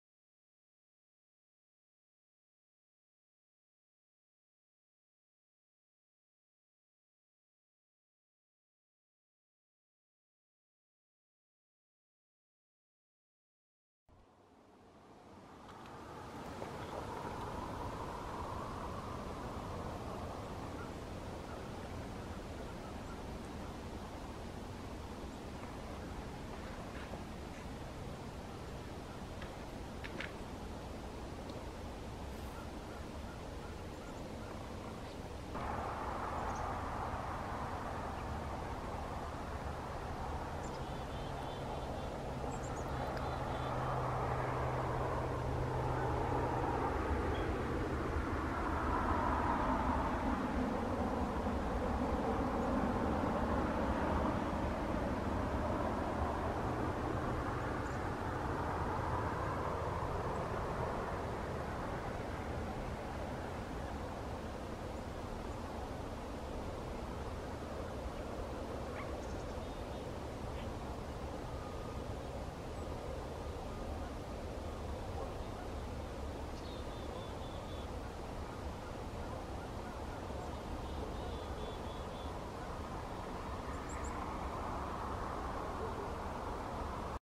Otters at Salmon Arm
Two River Otters spotted playing on the thin ice of a Beaver Pond along the Foreshore Trail in Salmon Arm BC  at about 9 am Dec1, 2020.